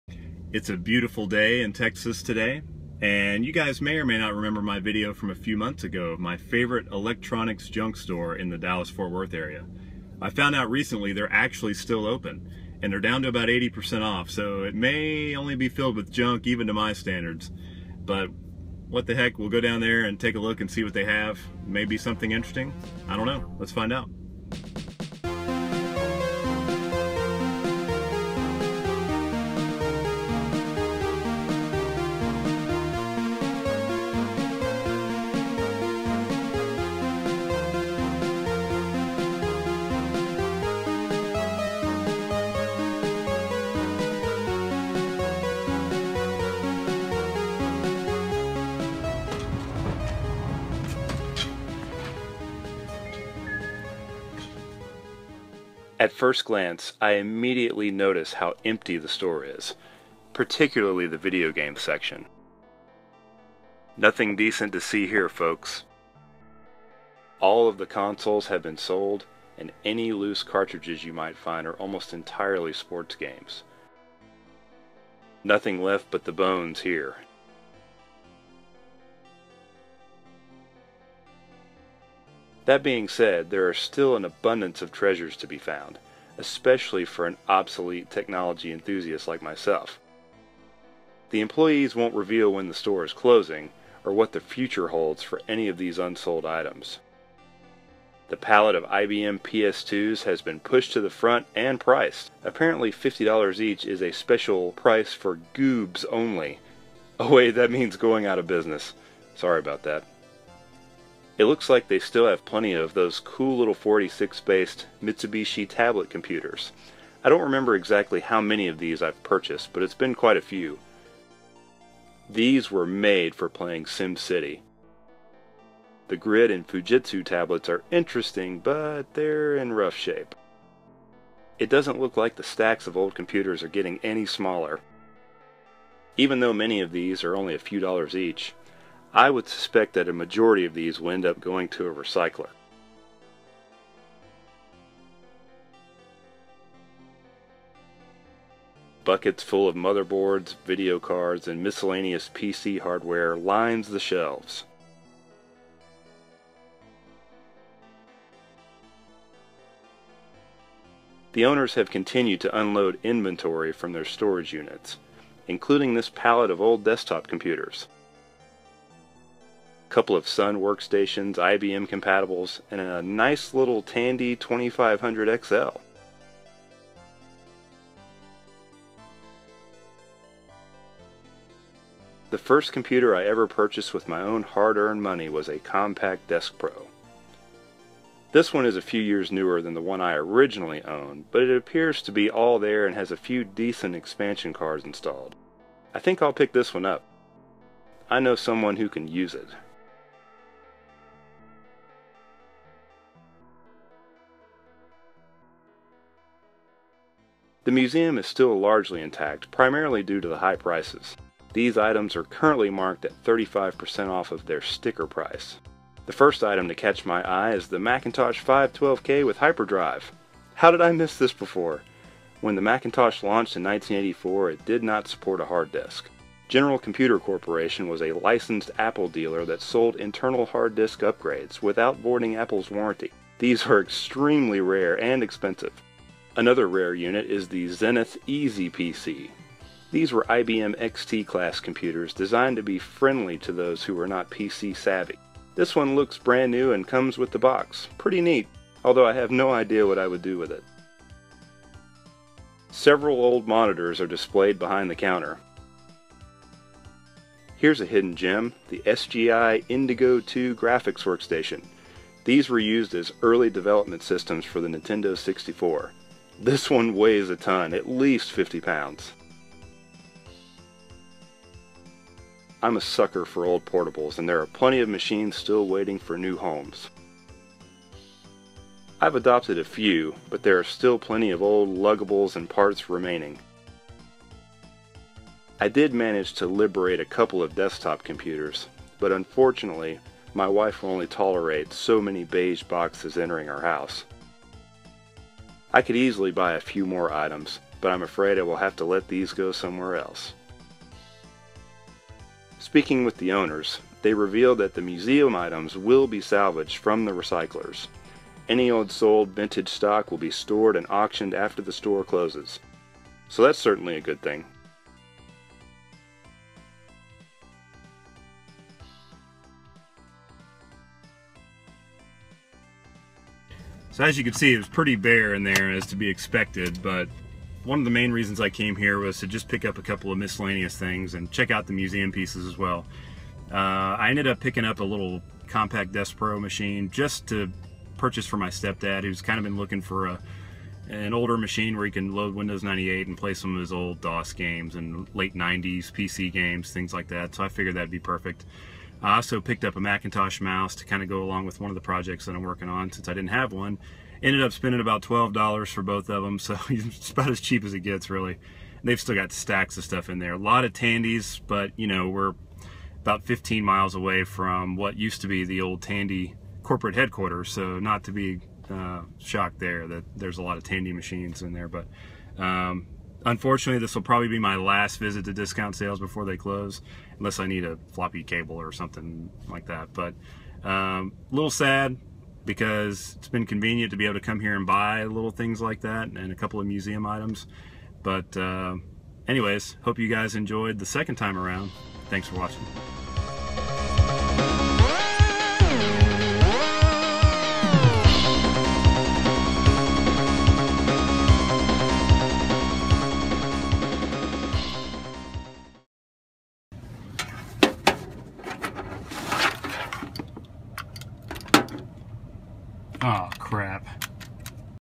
Computer Hunting Ep2: Return of the Junk Store - Obsolete Geek
Let's take a trip back to my favorite electronics watering hole!